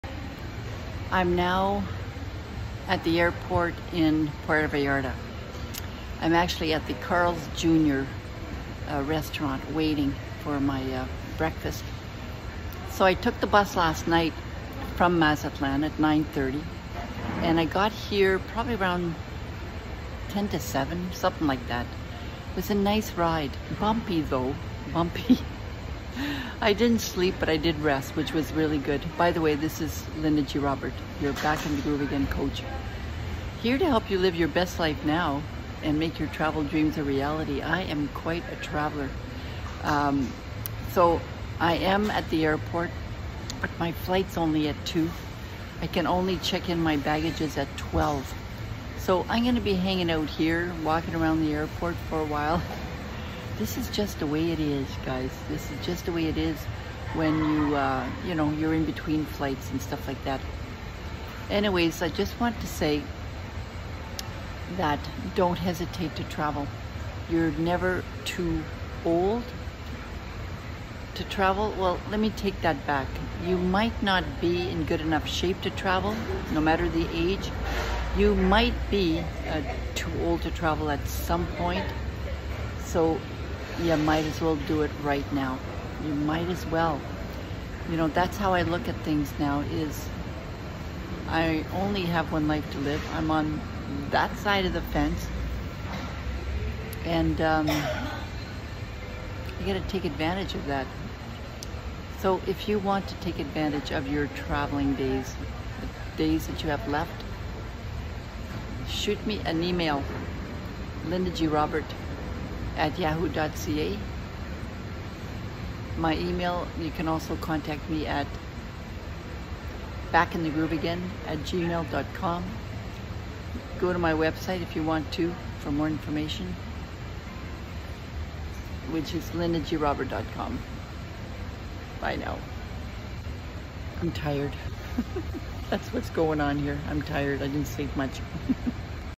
Waiting in Puerta Vallarta airport
In this video, I talk about the necessity to travel as you get older. You might be too old to travel one day as much as you can.